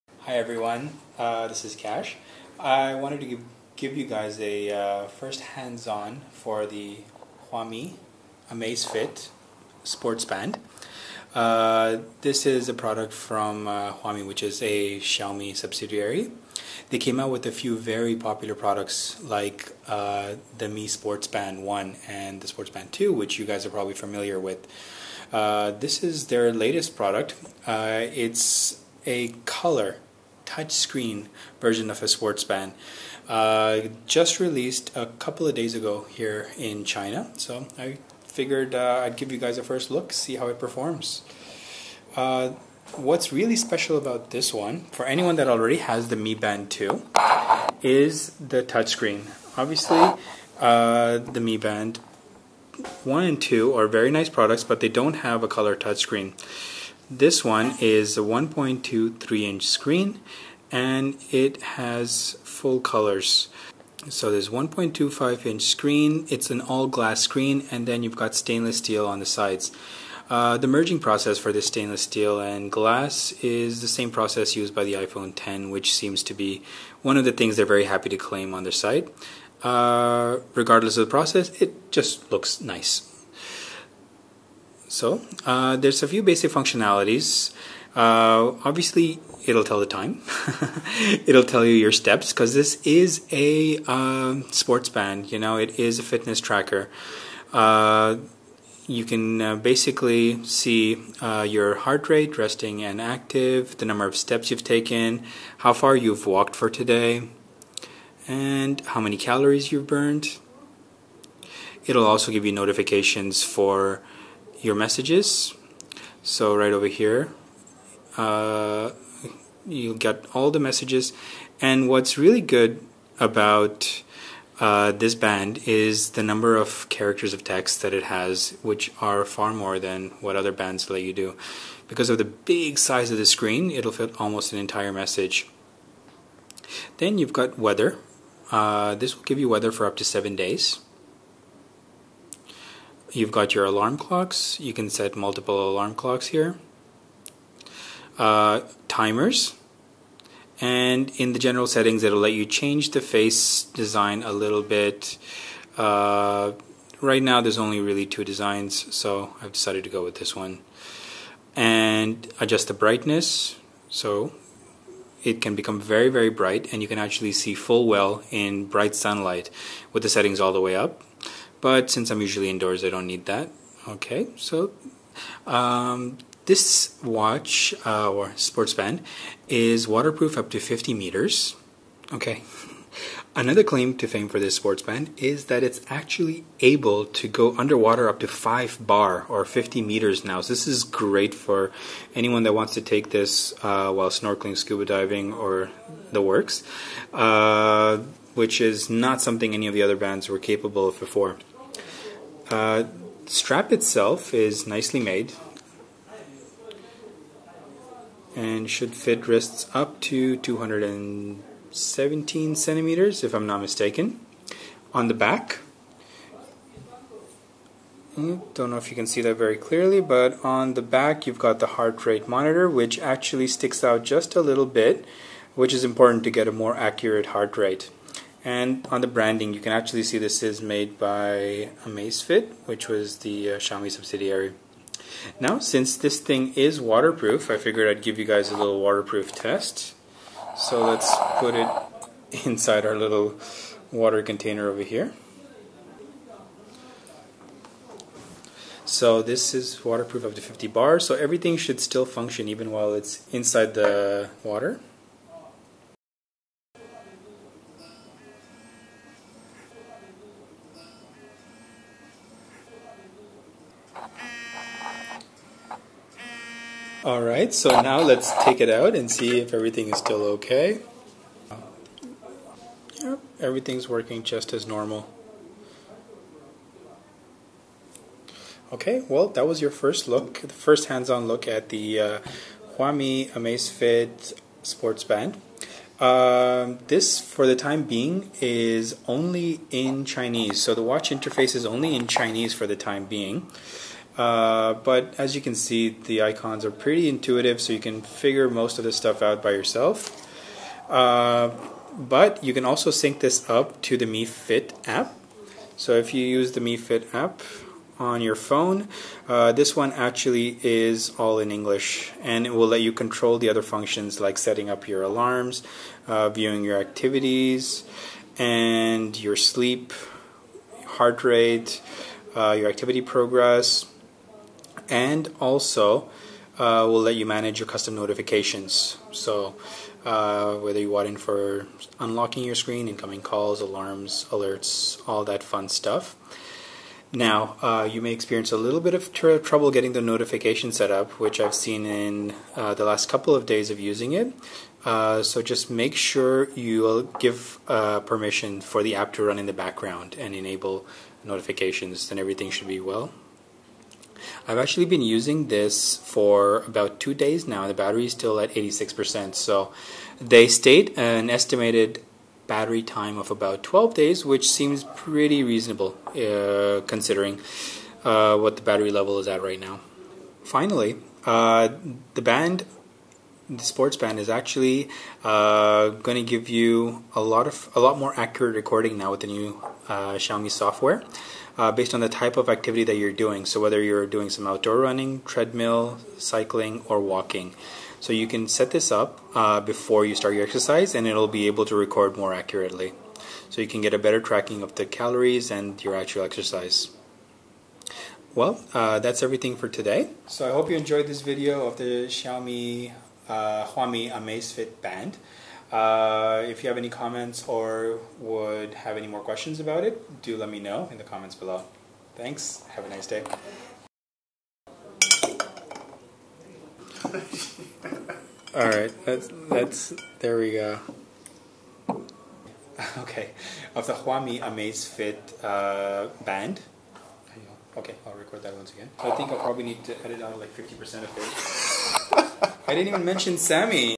NEWEST $45 Xiaomi Huami AMAZFIT Smartband Review by Kash
This is a hands on look at the new Amazfit Fitness Band created by Huami (a Xiaomi partner company.)
This fitness band is a successor to the Xiaomi's Mi Band 2 and the main improvements are a 1.23 inch IPS color touchscreen made from Corning Gorilla Glass 3, and 50 meters waterproofing.

The screen is bordered seamlessly with 316 stainless steel.

Product Parameters:
Screen Size : 1.23 inches
Type: color wide viewing angle IPS LCD
Resolution 80x160 RGB
Panel: 2.5D Corning gorilla glass 3 generation + AF coating
Sensor: triaxial acceleration sensor

PPG heart rate sensor
Bluetooth 4.1 BLE
Life time of
Battery life: ~12 days (100 per day notice, lift the wrist 50 times, running 1 hour per week)
Battery: 170mAH lithium polymer